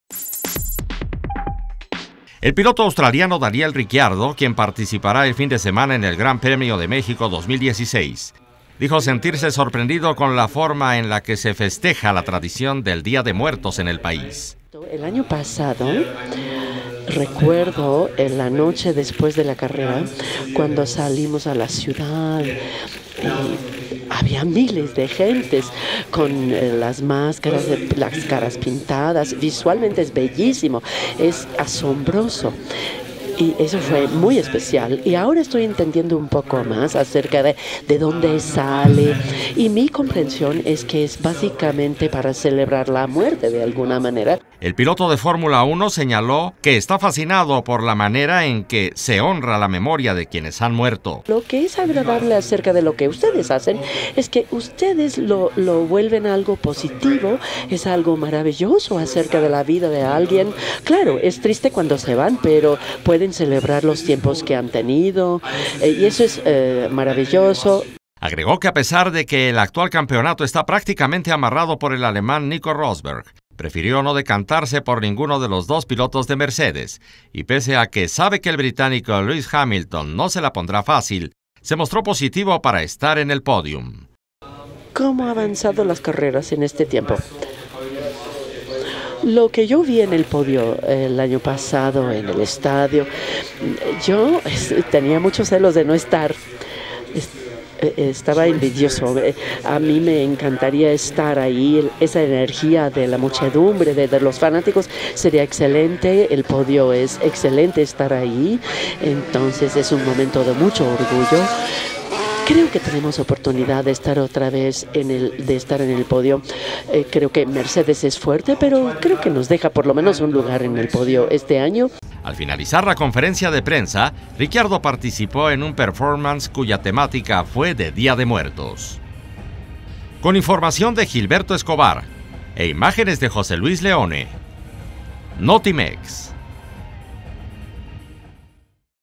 Sorprende a Daniel Ricciardo "Día de muertos" en México
El piloto australiano participará en el gran Premio de F1, el próximo fin de semana en el Autódromo Hermanos Rodríguez